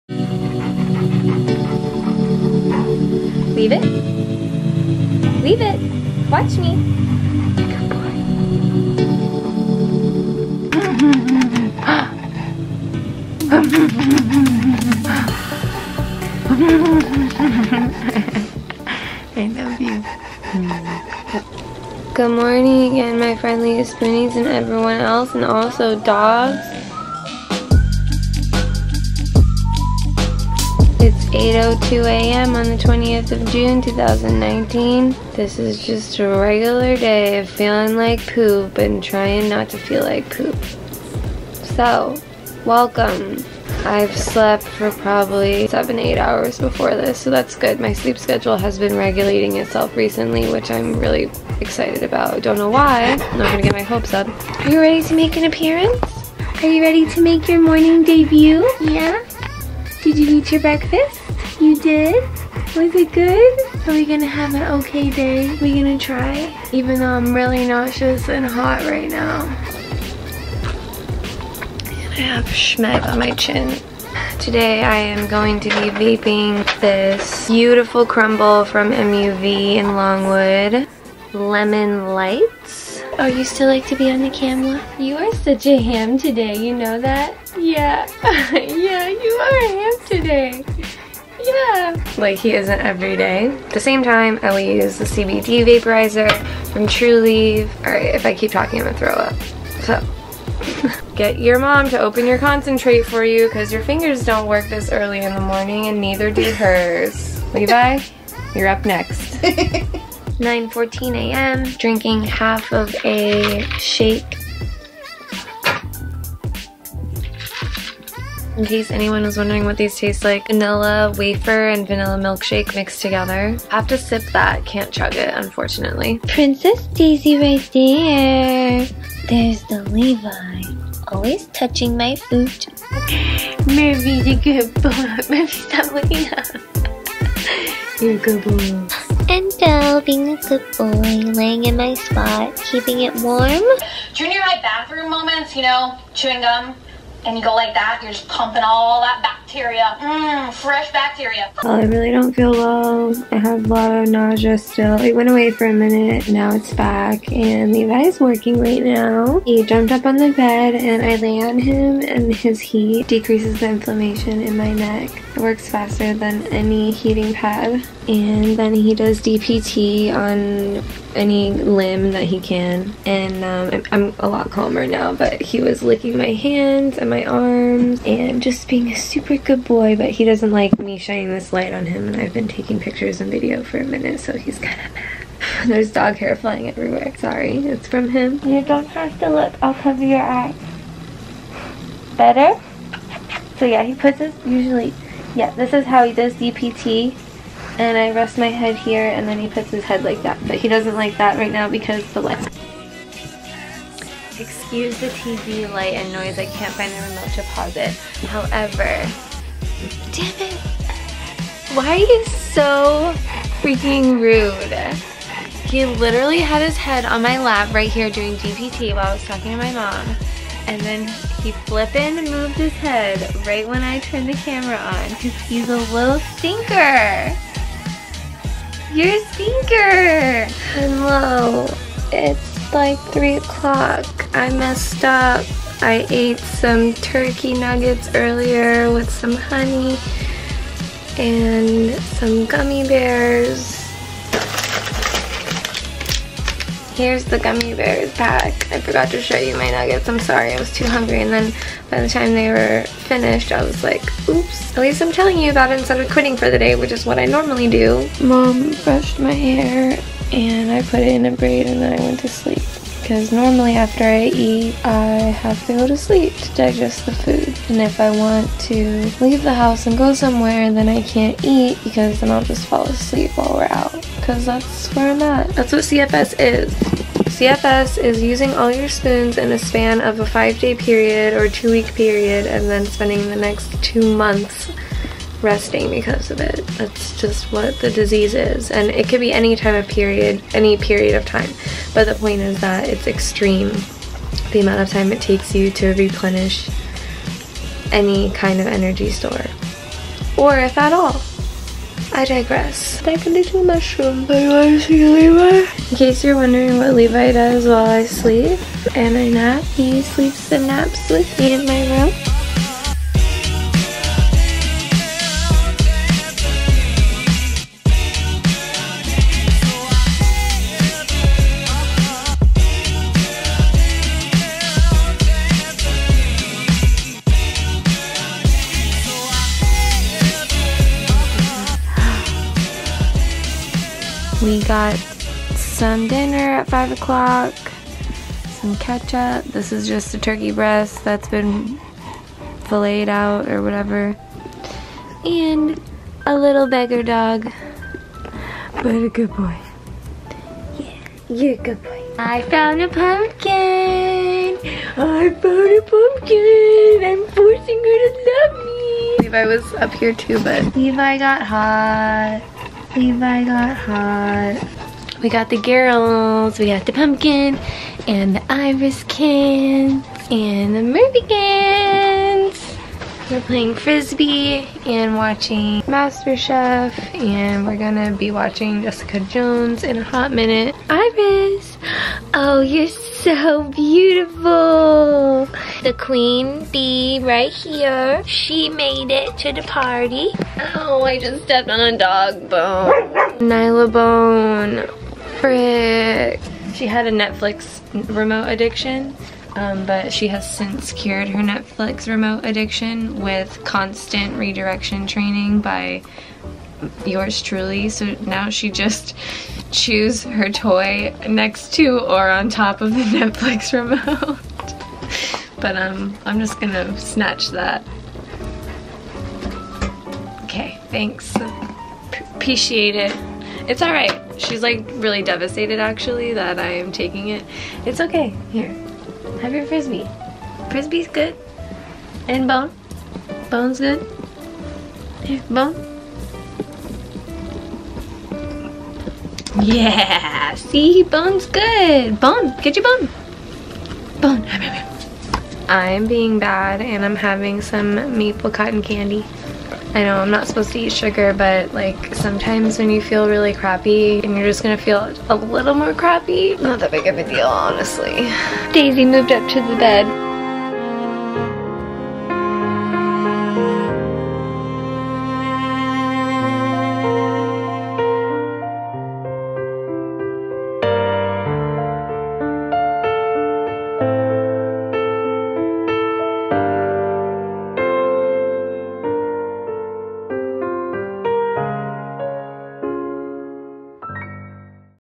housebound days with MECFS + a service dog
This video is meant to show that if I did a daily vlog, an average homebound day would look something like this.

Why do I make videos? I aim to grow authentic awareness of the millions missing from their lives due to the debilitating symptoms of mental illness and myalgic encephalomyelitis/chronic fatigue syndrome.

Let's be frens!